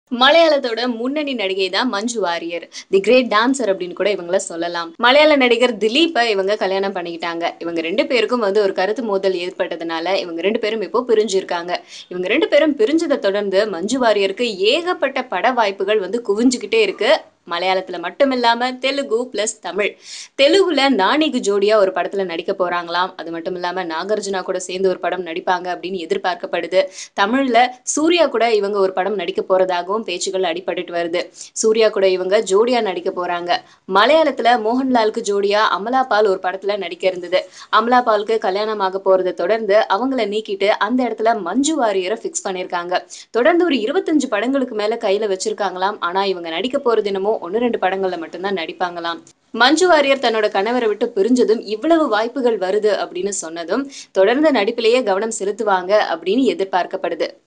Manju Warrier approached for acting opposite Surya | Next Movie | Hot Tamil Cinema News
Sources close to the actress say that she has as many as 25 offers to decide from various languages including Tamil, Telugu and Malayalam. The actress isn't rushing it up and is taking her own time to listen to all the scripts. She has offers to star opposite Nani and Nagarjuna in Telugu and opposite Suriya in Tamil